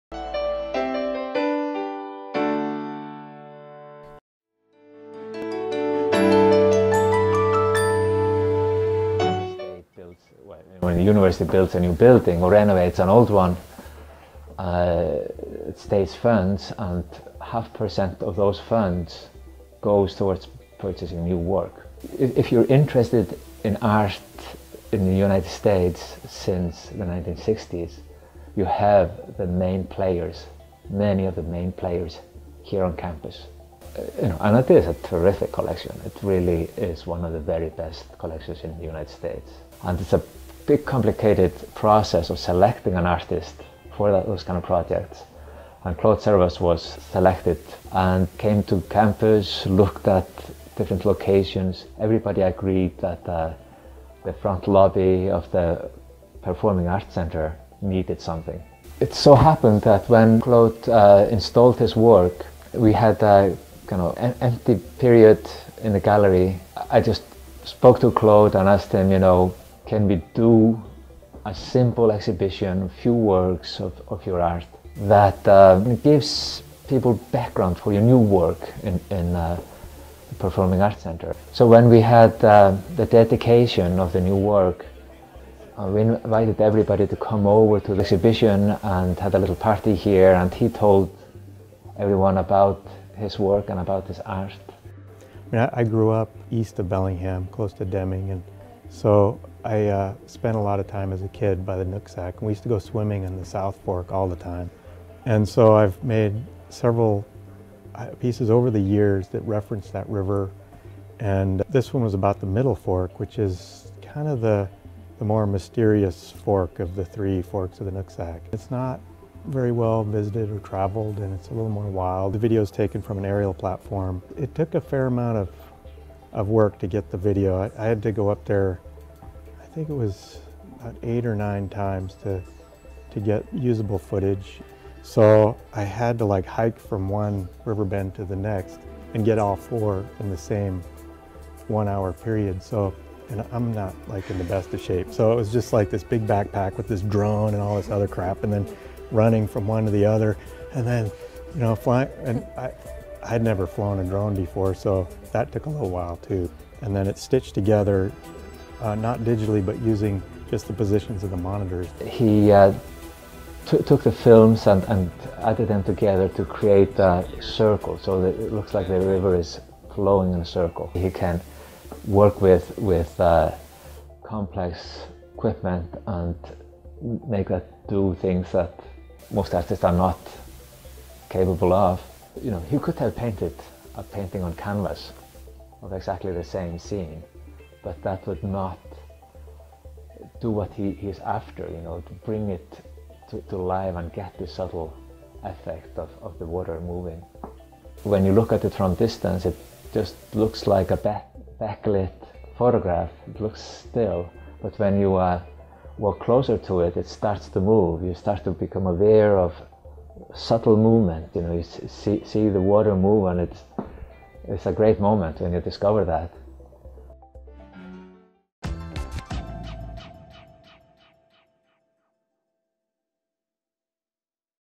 Claude Zervas sculpture Nooksack Middle Fork
Seattle based artist Claude Zervas (b. 1963) is best known for his light and video installations focusing on the north-west topography and topology. His video work in the PAC lobby shows aerial views of four bends in the Nooksack Middle Fork, which supplies water for the City of Bellingham via pipe and tunnel division into Lake Whatcom. Zervas used a drone to fly his video equipment so he could capture peaceful images of the river flowing through the forest. The video segments are shown on four monitors arranged in a rough circle, giving, in Zervas' words, an impression of  “an impossible circular river that flows back into itself indefinitely.” Quoting the ancient Greek philosopher Heraclitus famous dictum, “Everything flows,” Zerves adds “and so will this river.”